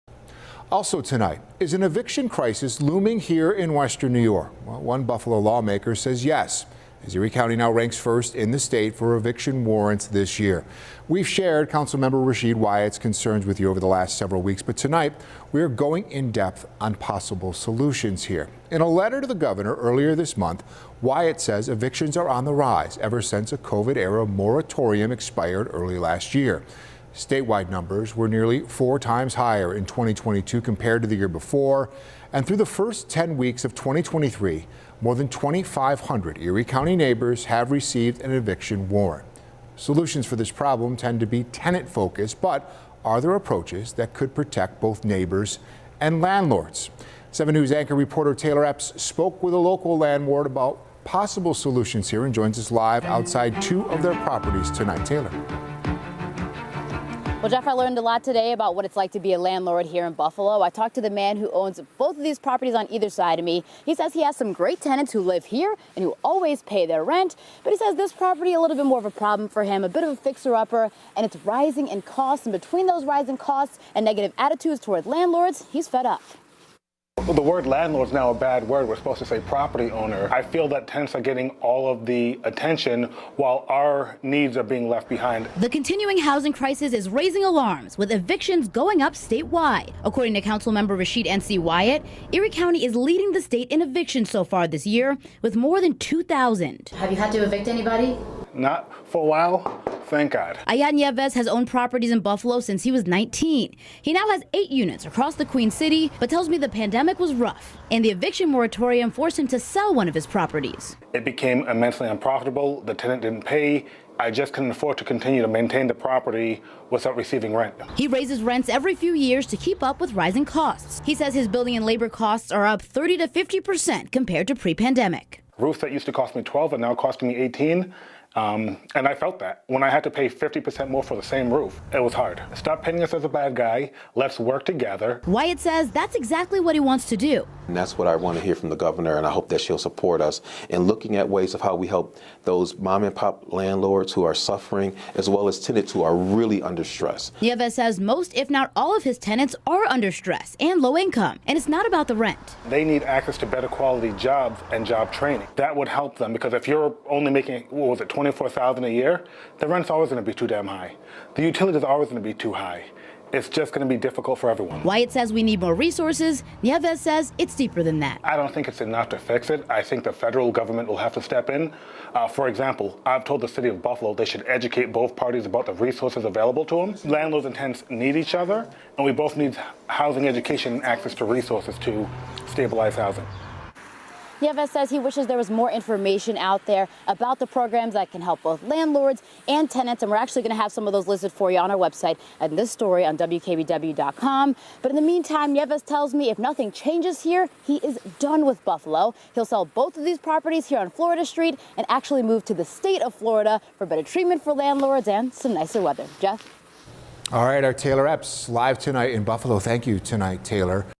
Buffalo landlord on evictions: "Stop painting us to be the villain"
Evictions are processing every day in Buffalo City Court at a rate that's ringing alarm bells for one Buffalo Common Council Member.